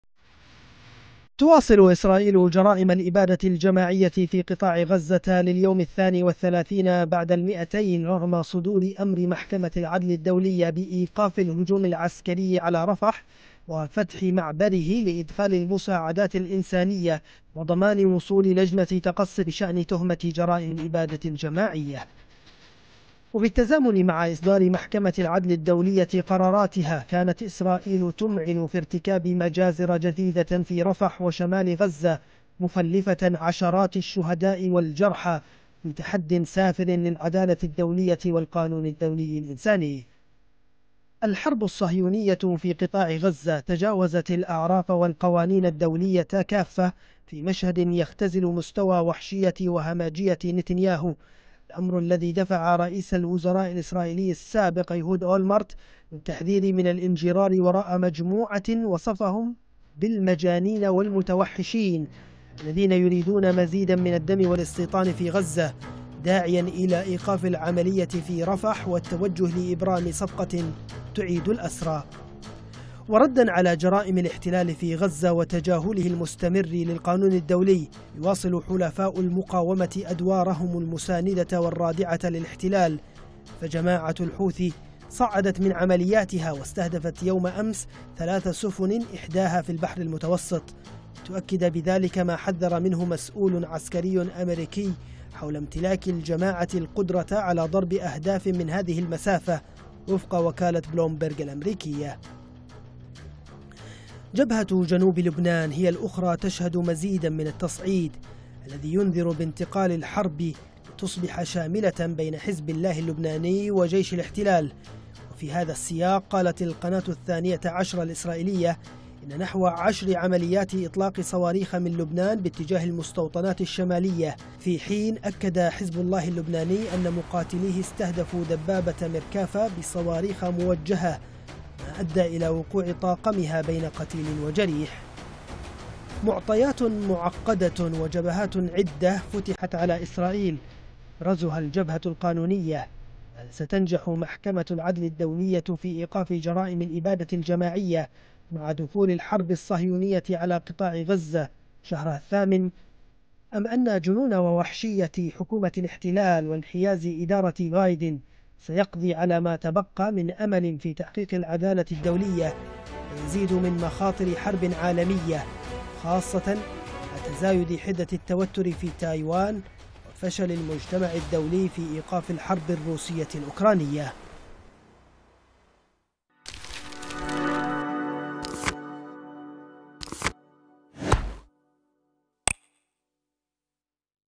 إسرائيل في قفص العدالة الدولية وصـ ..واريخ الحـ . ـوثي تصل البحر المتوسط
إسرائيل في قفص العدالة الدولية وصواريخ الحوثي تصل البحر المتوسط